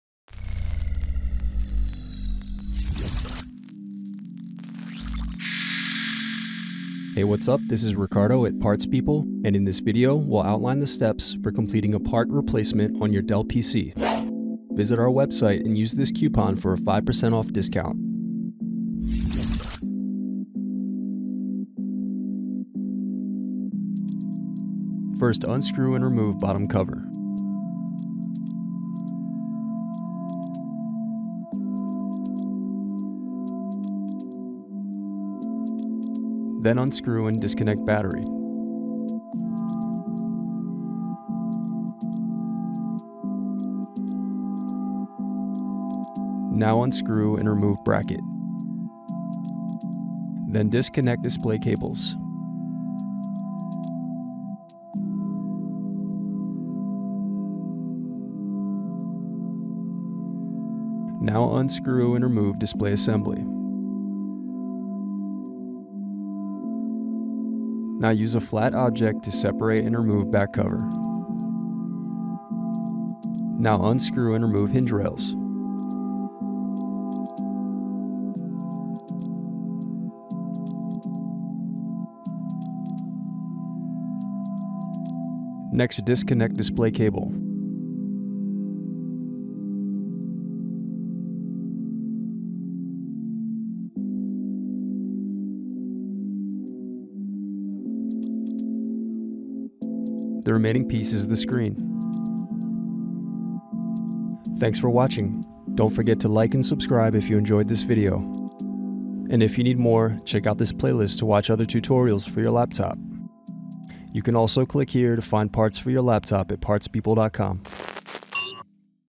Dell Latitude 3190 2-in-1 (P26T003) LCD Panel Replacement Video Tutorial
This Dell Latitude 3190 2-in-1 video repair tutorial was brought to you

Shop for more than 250,000 Dell parts shipped the same day